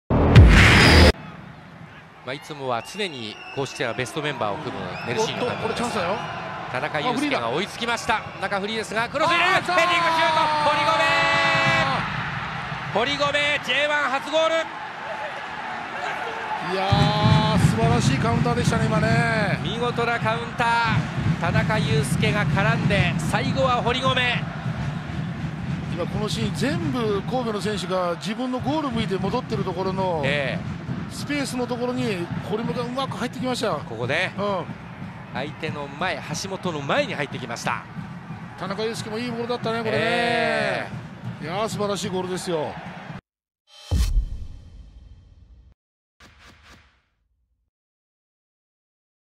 【公式】ゴール動画：堀米 勇輝（甲府）38分 ヴィッセル神戸vsヴァンフォーレ甲府 明治安田生命Ｊ１リーグ 第9節 2017/4/30
左サイドで切り返した味方のクロスに堀米 勇輝（甲府）が飛び込み、J1初ゴールで先制点が決まる！明治安田Ｊ１のゴール動画を公開中！
2017年4月30日（日）に行われた明治安田生命Ｊ１リーグ 第9節 神戸vs甲府のゴール動画です。